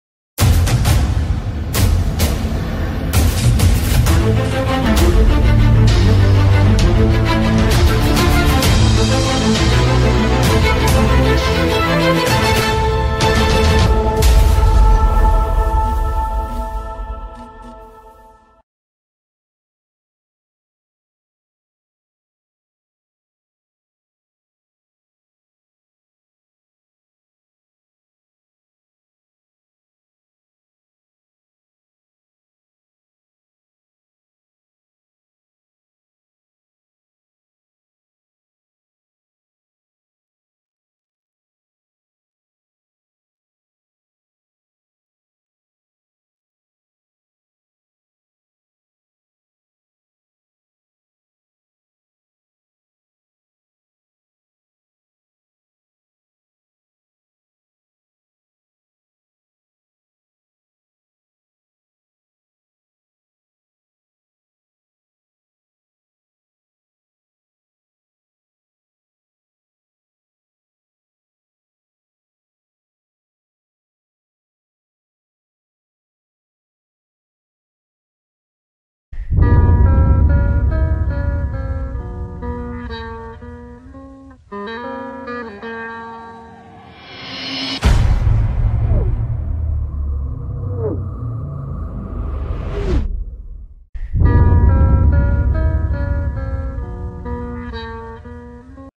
Damion Jeanpiere Jr , WR, Nicholls State | 2019 NFL Draft Highlights | Discovering Diamonds
Damion Jeanpiere Jr. from Nicholls State has a ton of swag and I love his big play ability. This year Jeanpiere Jr. dominated his division and was tough to cover. The Nicholls State wideout will try to show the NFL scouts that he has what it takes in the coming months. Remember his name because he is special.